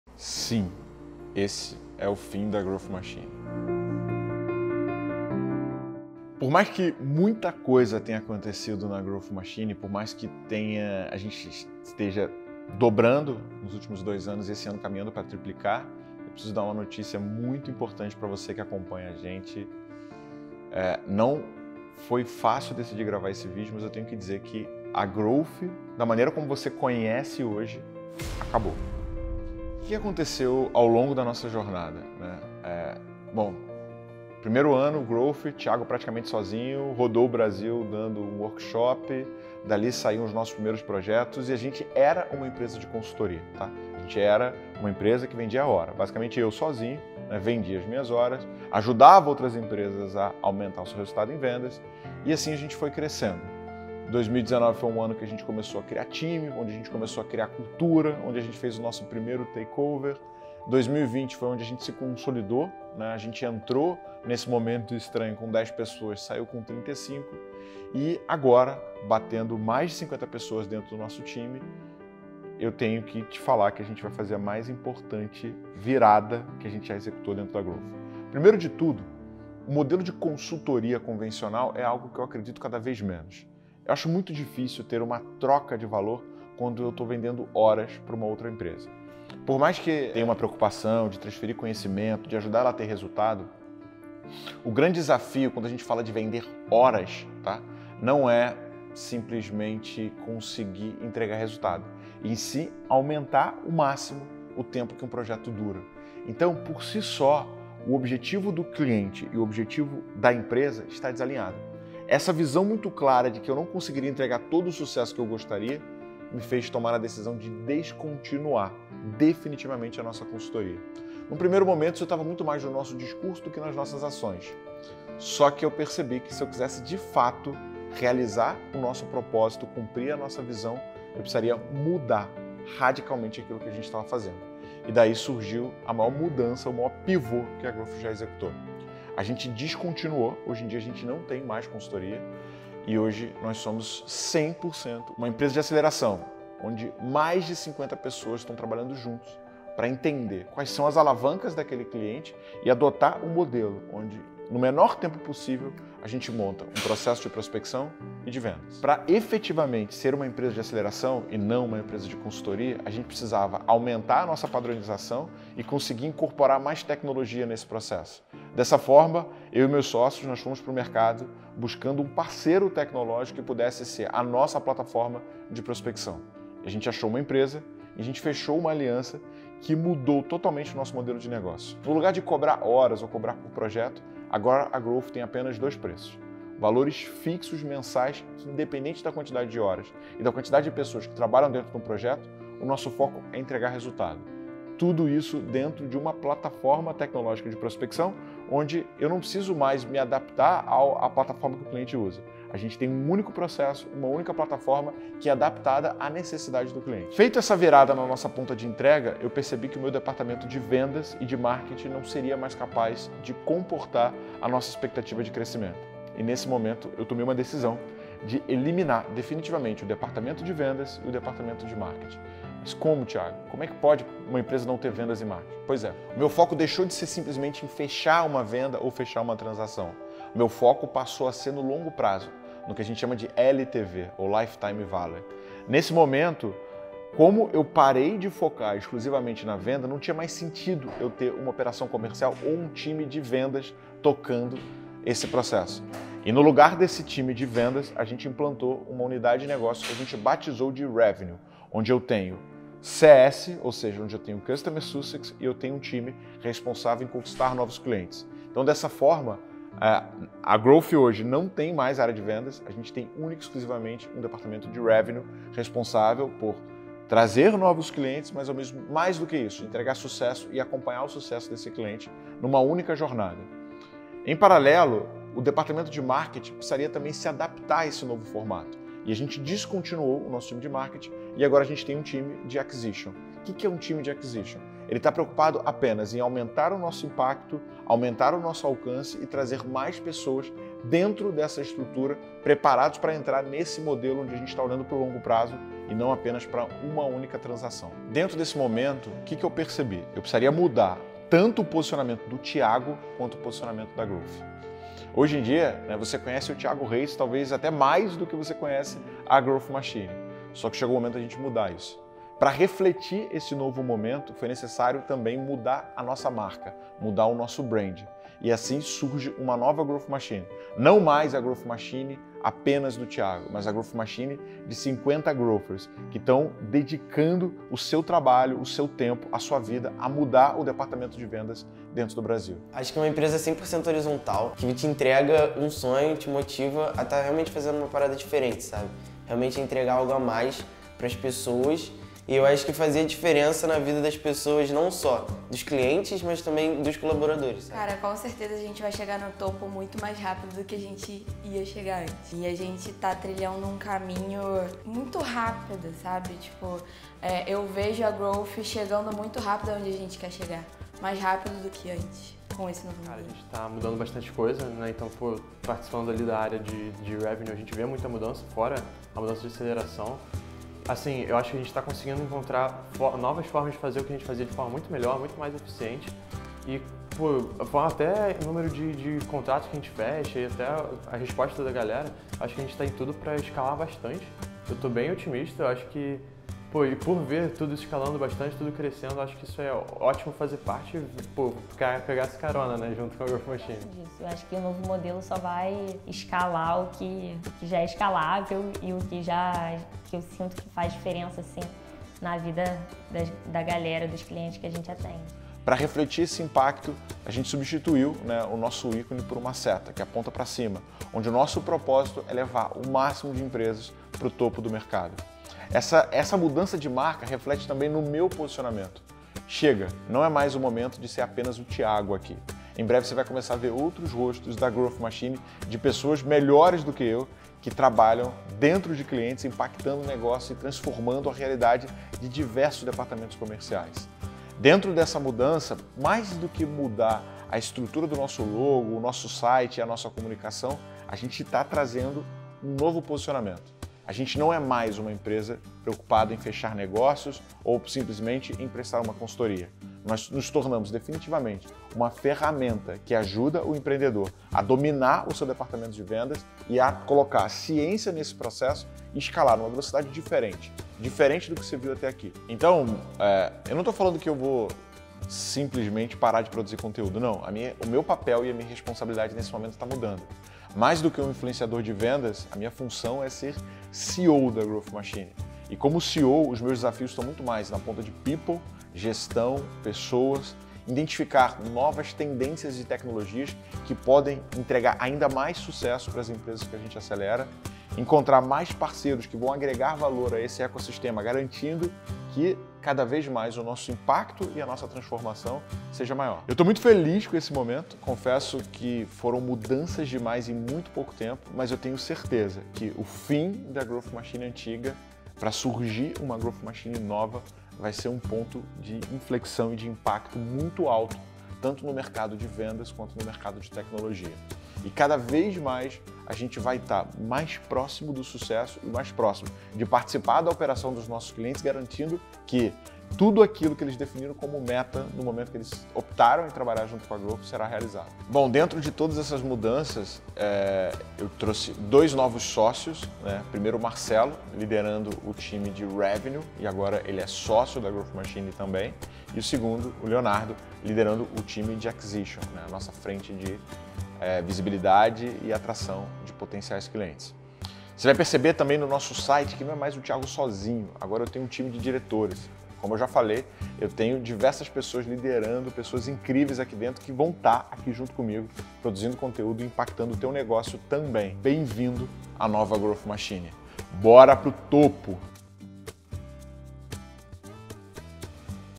O fim da Growth Machine
Chegou o nosso fim.

Mais uma empresa afetada por esse momento difícil?

No início do ano eu e meus sócios entendemos que o modelo de trabalho que estávamos executando não era sustentável.

Vendas de horas é algo que não se encaixa com o objetivo do cliente.

Ele quer resultado no menor tempo possível, a empresa de consultoria quer vender o projeto. Enquanto o cliente fica ansioso para ver sua receita aumentar, a consultoria busca tornar o projeto o mais longo possível.

Certa vez ouvi de um CEO que admiro a seguinte frase: "Temos que pensar como matamos nosso próprio negócio, antes que a nossa concorrência o faça!"

Depois de muito refletir tomei uma das decisões mais difíceis da minha vida.

Encerrar meu ciclo na Growth como ela é hoje.

Abolimos definitivamente a consultoria, nossa principal fonte de receita.

Não estamos mudando apenas a nossa marca e nossa comunicação.

Mudamos tudo que fazemos para estarmos alinhados com o nosso propósito e a nossa visão.

No lugar da consultoria, simplificamos tudo! Temos apenas dois planos, que não tem ligação com horas do projeto e sim com o número de pessoas em vendas.

Com a tecnologia e o nosso método, entregamos a máquina de vendas 100% montada e pronta para funcionar em apenas 30 dias.

Nesse momento, estamos lançando oficialmente nossa ferramenta de prospecção que trabalha 100% integrada com nosso método de aceleração de vendas, para fazer uma única coisa:

Te levar para o topo!

Bem vindo a nova Growth Machine!